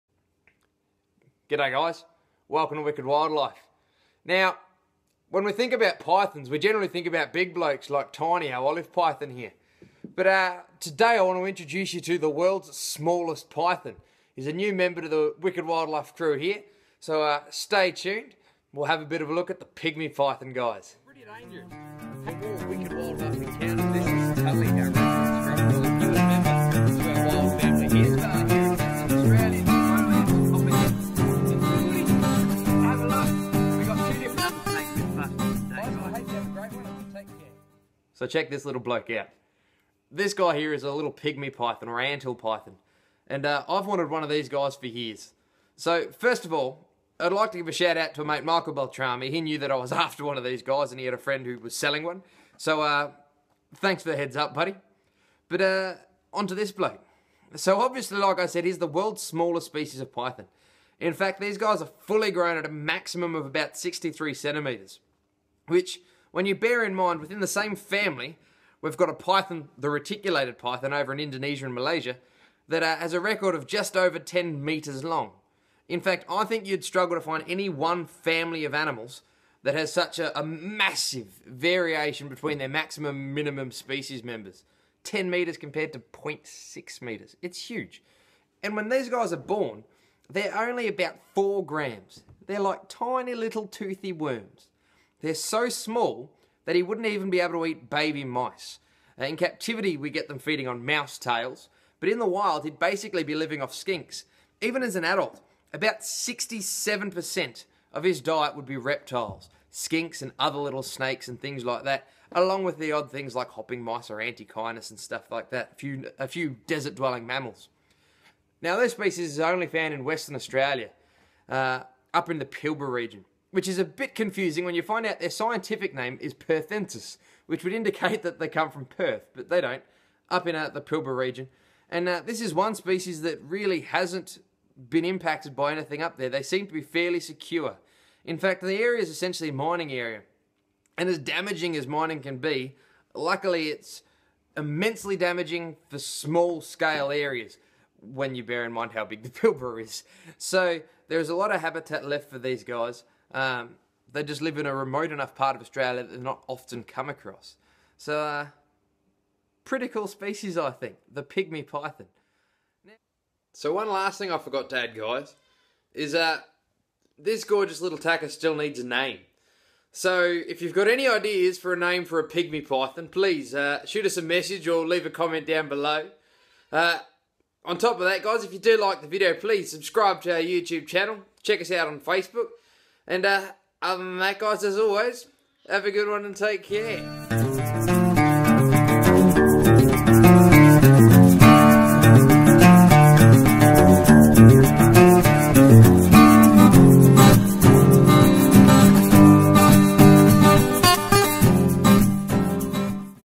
Worlds Smallest Python - Help Name Our Pygmy Python
When we think of pythons we often think of the big guys, burmese pythons, reticulated pythons, event olive and scrub pythons, but Australias West Coast is home to the worlds smallest python, the Pygmy or Anthill Python